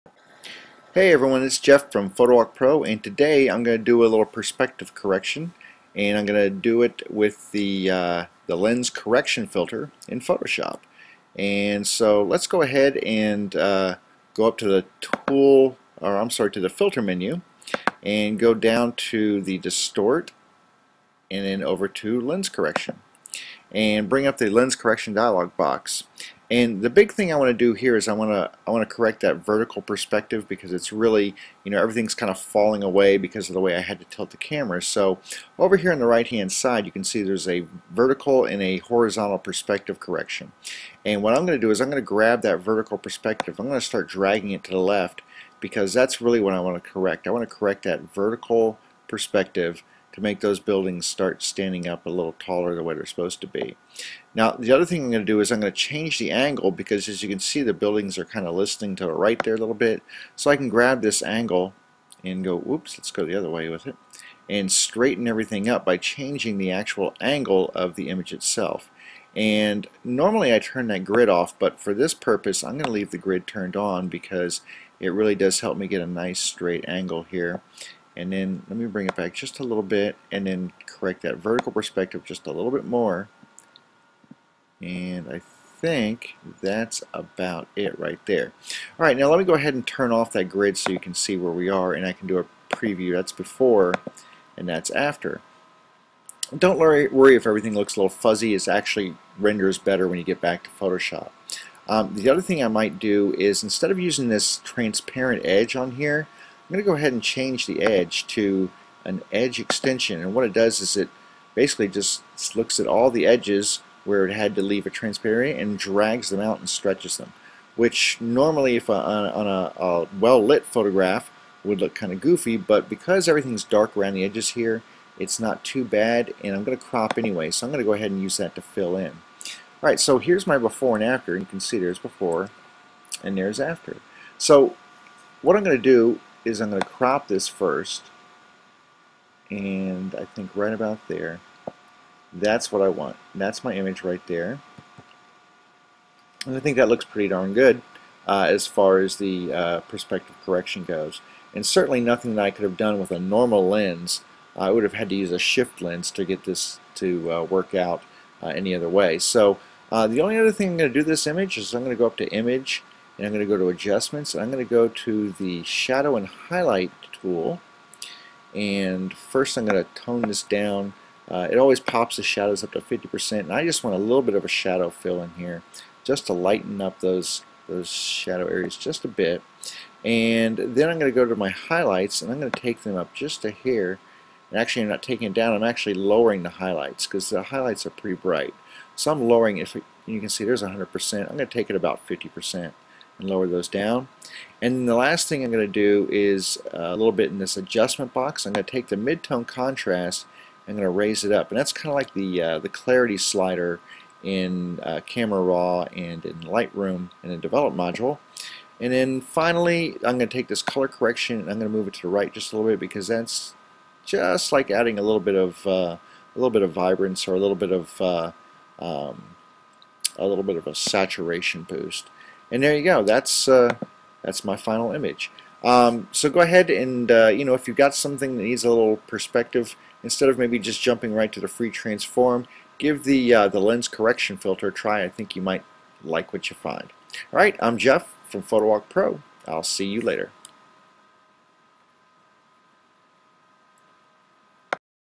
Perspective Correction in Photoshop
Using the Lens Correction Filter in Photoshop to do some perspective correction.